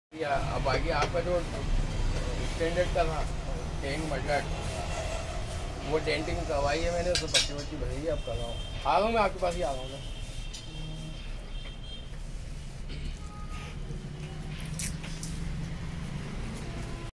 Imported paint mixing for Yamaha RD 350
Mixing paint for Yamaha Rajdoot RD 350. The whole process is done through computer to get the exact color mix and codes.